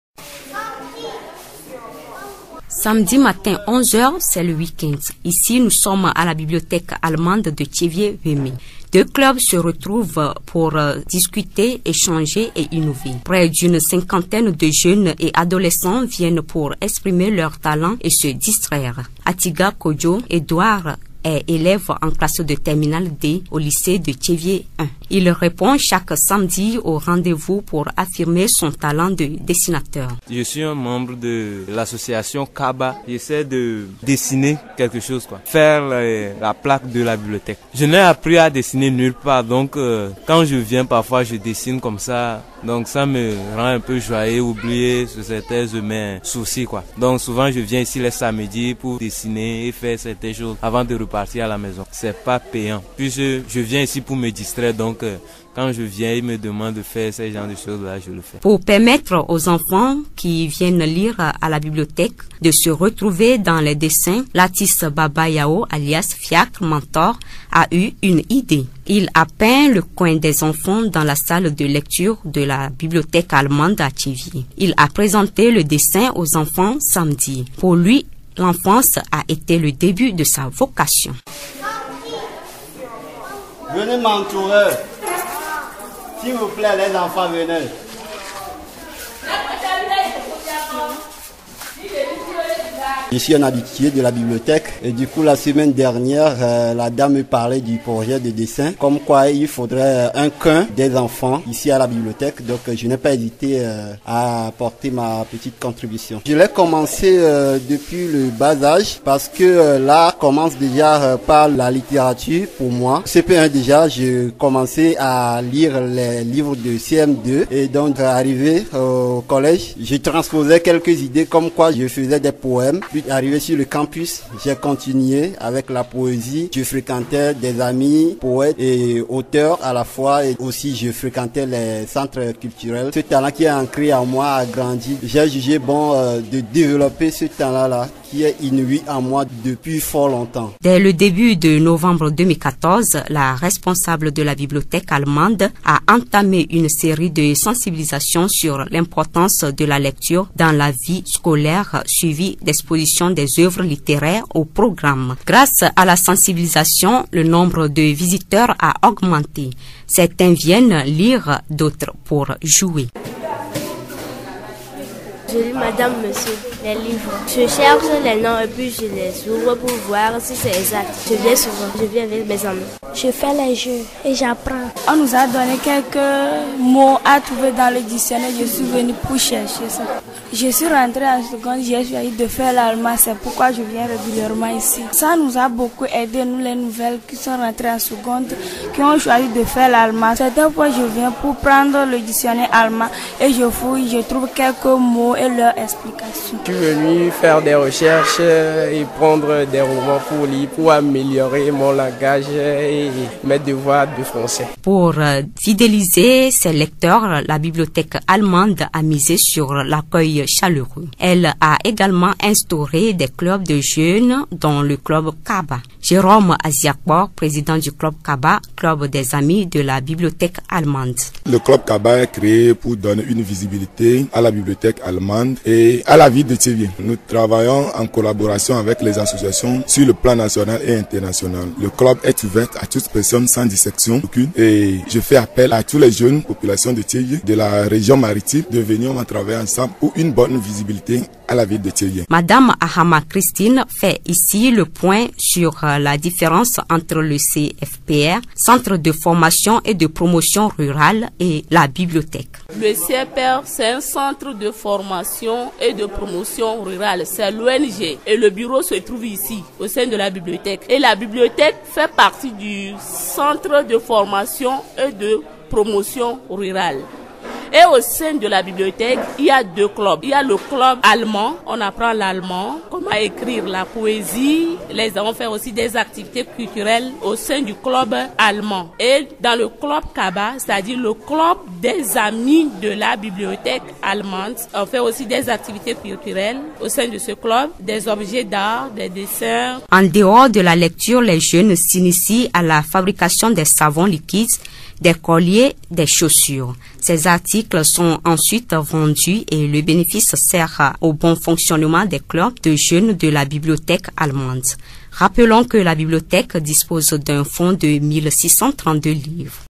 Bibliothèque Allemande de Tsévié Wémé Visite Surprise
Blessing KPOSSOGNA rend visite à nos ami(e)s de la Bibliothèque Allemande de Tsévié Wémé.
Dans cet environnement, nous avons aussi bien l'odeur des livres dans les narines que les voix qui s'élèvent pour les lire dans les oreilles. L'odeur de la peinture se dissipe entre les 4murs de la Bibliothèque, Madame AHAMA Christine accueille chaque jour, les élèves, leurs parents et près d'une centaine de visiteurs.
Nous leur avons rendu visite, écoutons le Compte rendu de Blessing KPOSSOGNA...